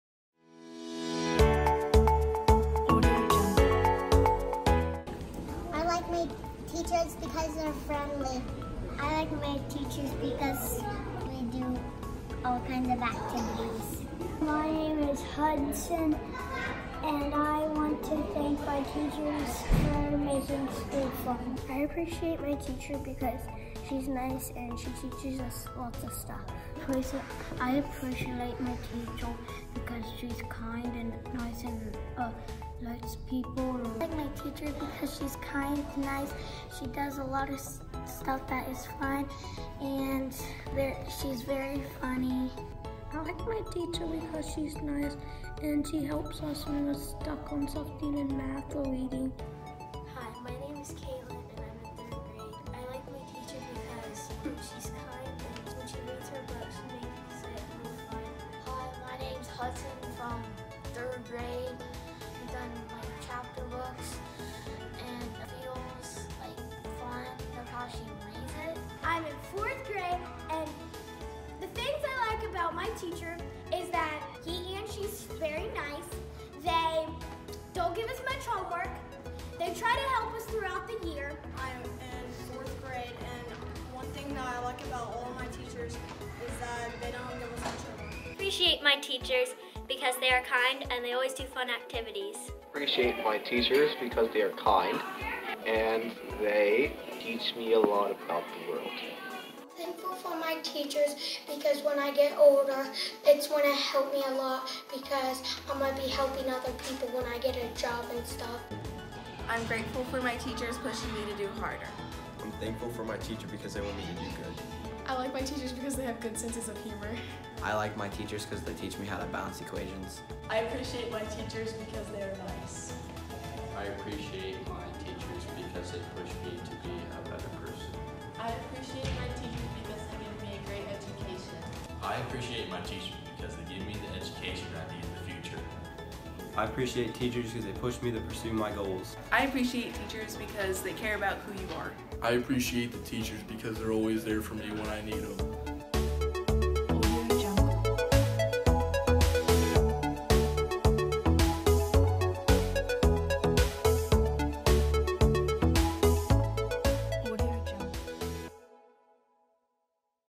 Why We Appreciate Our Teachers
Happy Teacher Appreciation Week to all of the staff at Adena Local Schools.  You are all greatly appreciated.